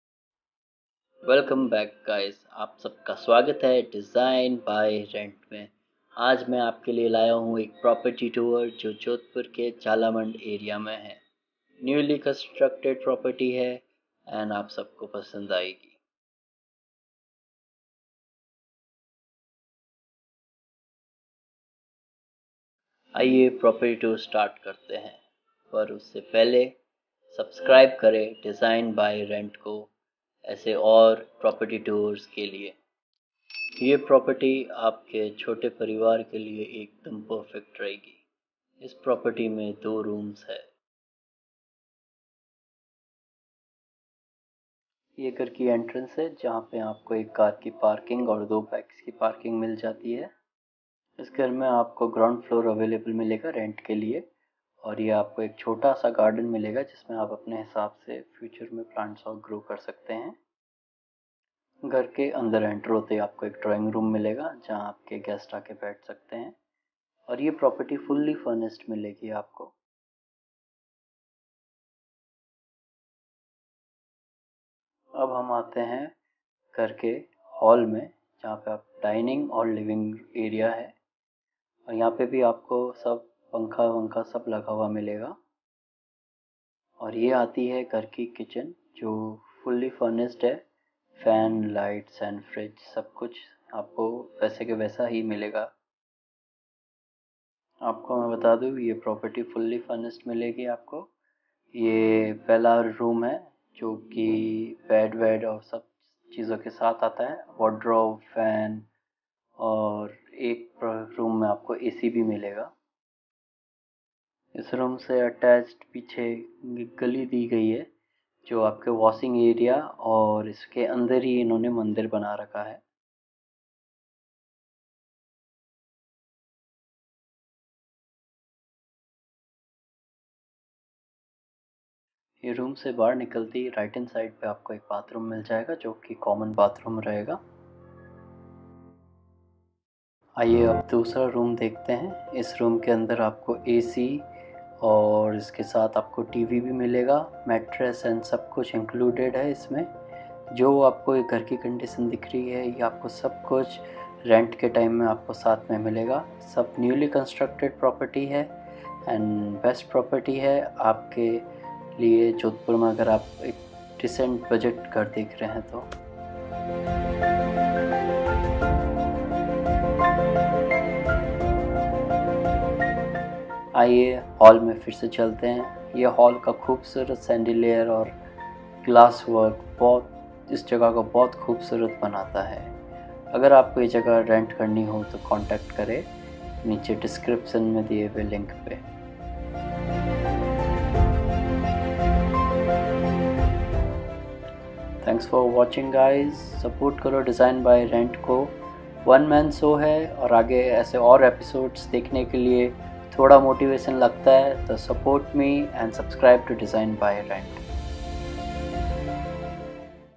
Jhalamand house for rent | Newly constructed | Jodhpur (Rajasthan) | Tour 3
Newly constructed house available for rent with fully furnished set up near Jhalamand circle, near new high court Jodhpur.
feel free to drop what's app or call us.